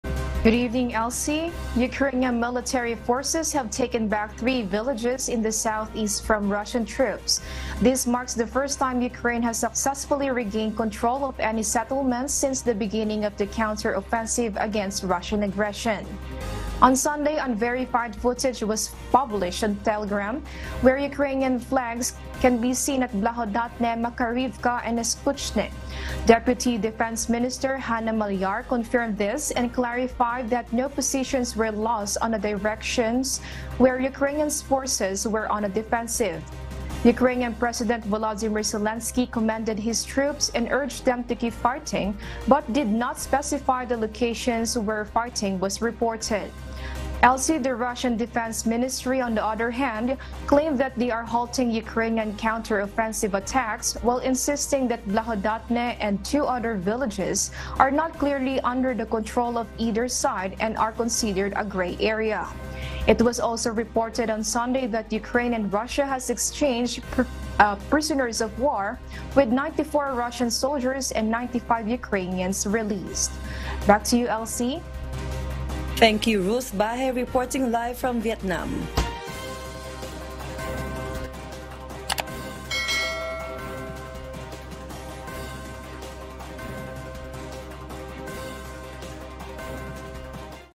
Ukraine hails efforts of counteroffensive against Russia
Ukraine celebrates the initial results of its counteroffensive and asserts the recapture of several villages in the eastern Donetsk region.